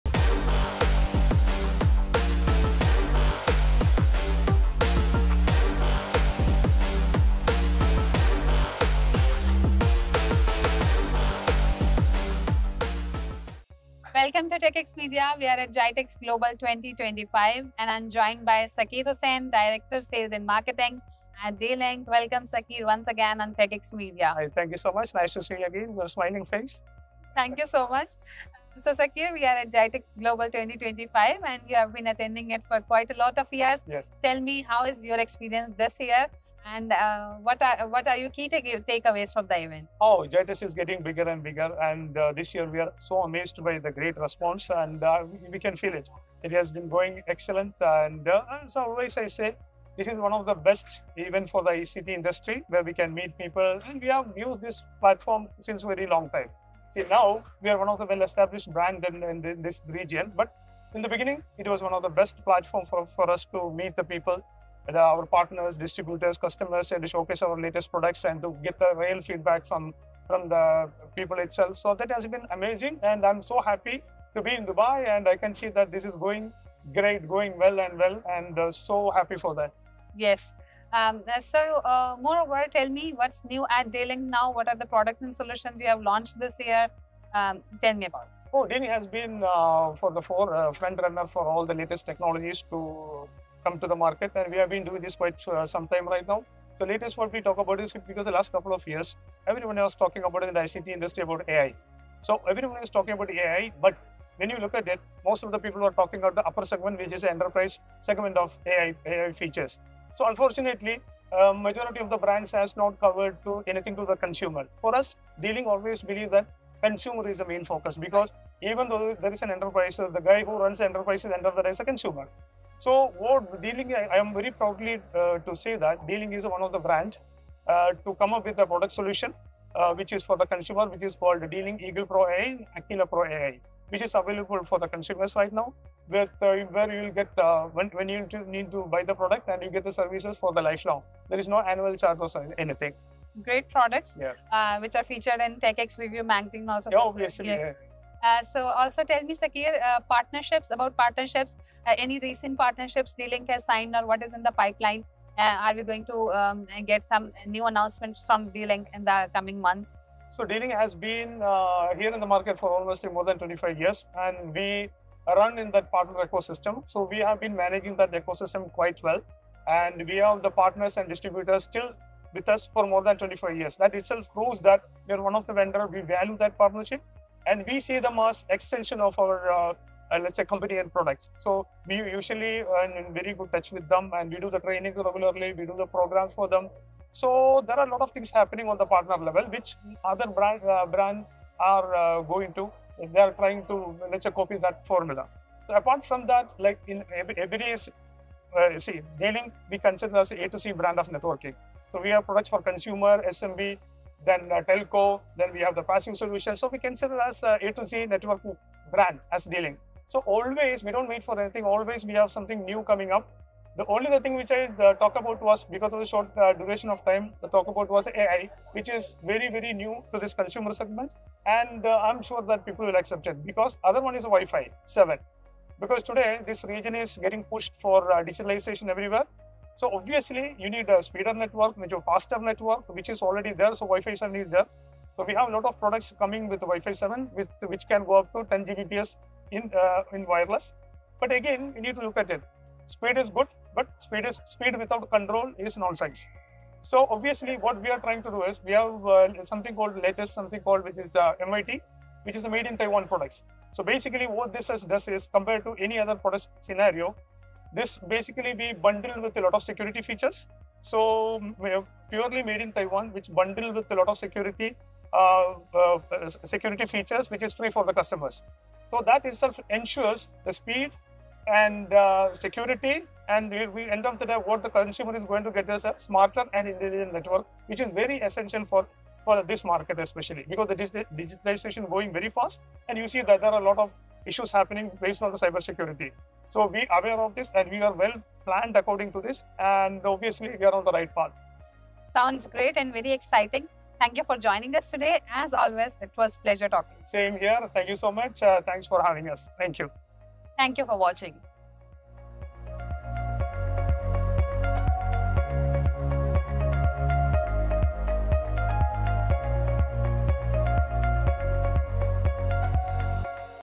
How D-Link is Powering Smarter Connected Homes | Sakkeer Hussain | GITEX Global 2025
At GITEX Global 2025, TECHx Media caught up with Sakkeer Hussain, Director of Sales and Marketing at D-Link Middle East, to discuss the company’s latest innovations, partner ecosystem, and how D-Link is redefining consumer networking with AI and Wi-Fi 7 technologies.

Interview Highlights:
🟩 D-Link’s long-standing success at GITEX and its regional growth journey
🟥 Introduction of Eagle Pro AI series bringing artificial intelligence to home users
🟦 Focus on consumer-first innovation with lifetime service and zero annual fees
🟨 Strong partner ecosystem built over 25 years, emphasizing trust and collaboration
🟪 Launch of Wi-Fi 7 products offering up to 10 Gbps speeds with advanced security features
🟩 New Made in Taiwan lineup bundled with built-in cybersecurity protection
🟥 D-Link’s commitment to building smarter, more secure, and reliable digital networks